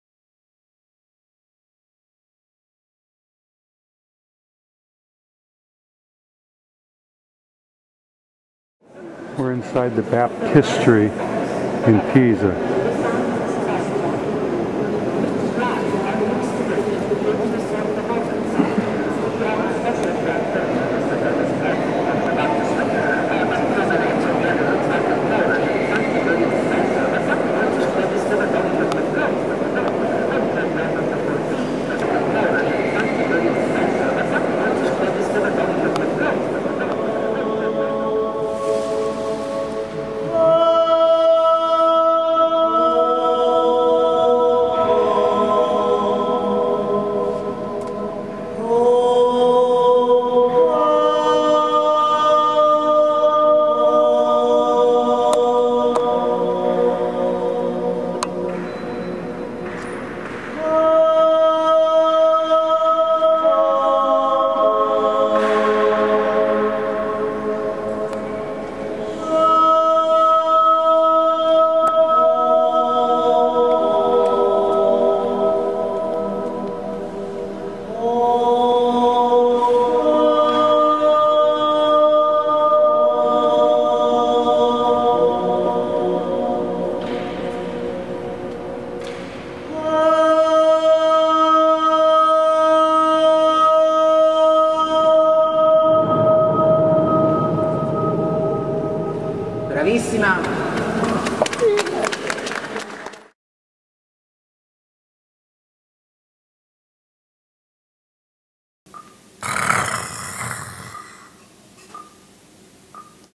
Singing in the Baptistry in Pisa, Italy
This is a video of a woman singing in the Baptistry in Pisa, Italy in August 2008. The voice echoes all around the building with only one voice singing. It really is quite an experience to hear it. The Baptistry is one of the 3 buildings in theSquare of Miracles in Pisa, Italy. The other two are St. Mary of the Assumption Cathedral and the Belltower, more commonly known as the Leaning Tower of Pisa.